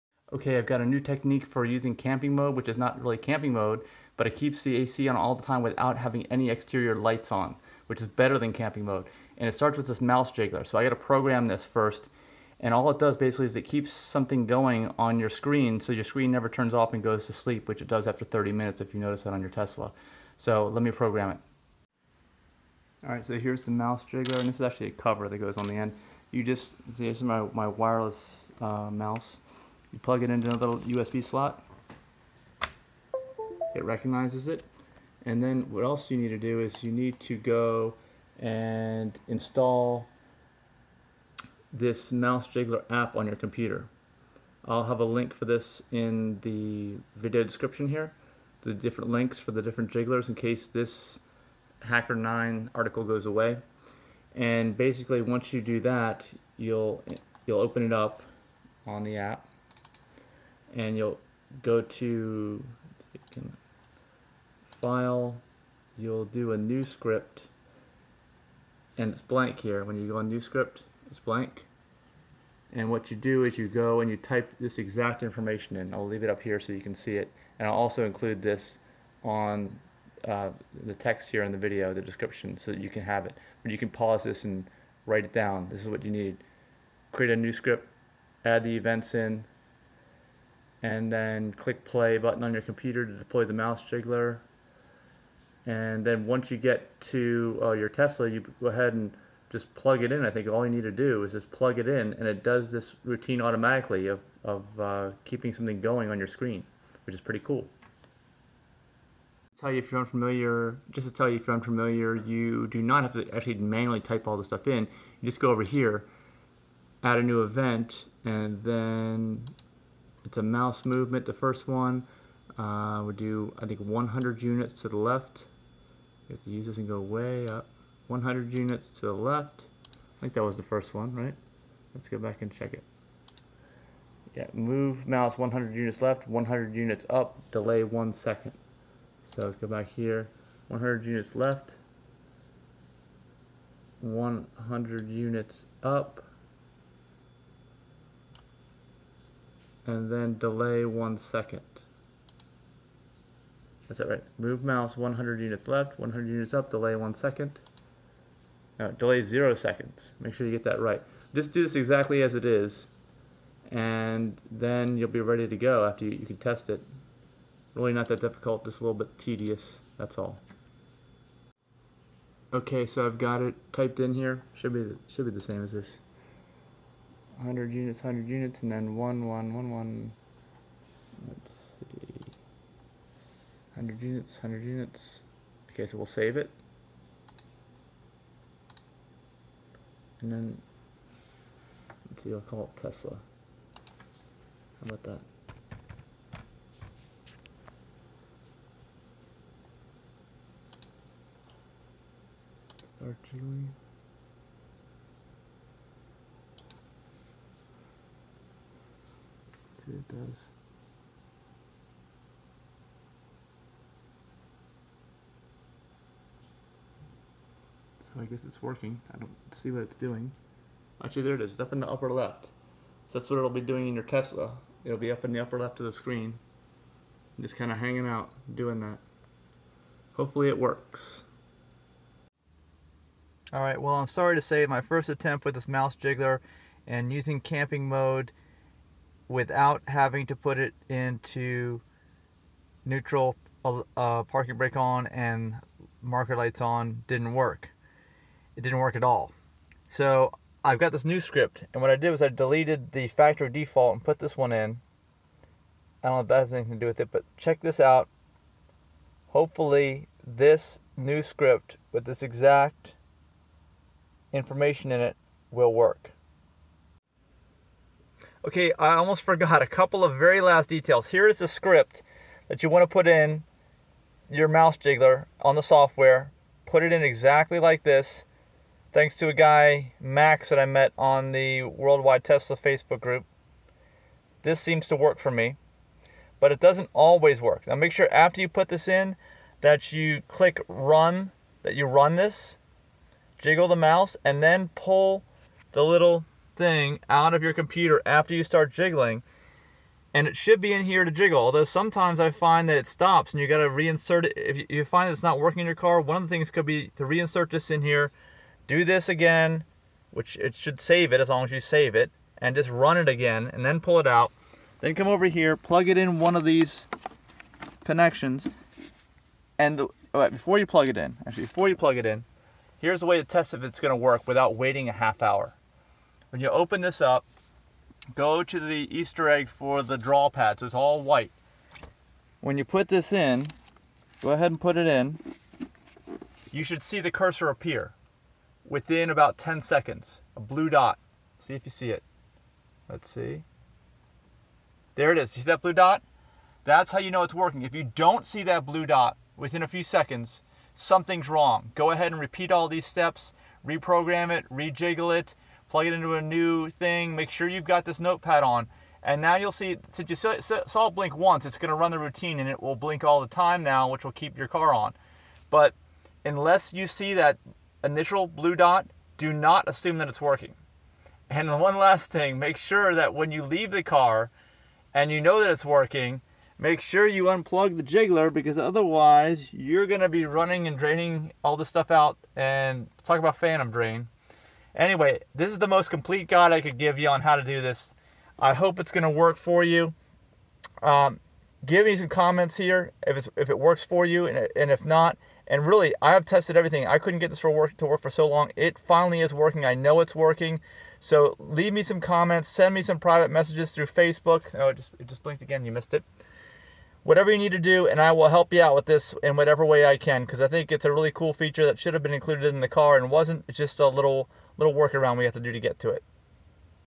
Tesla Camp Mode WITHOUT Exterior Lights, an App, or Internet | Mouse Jiggler
The best way to do this!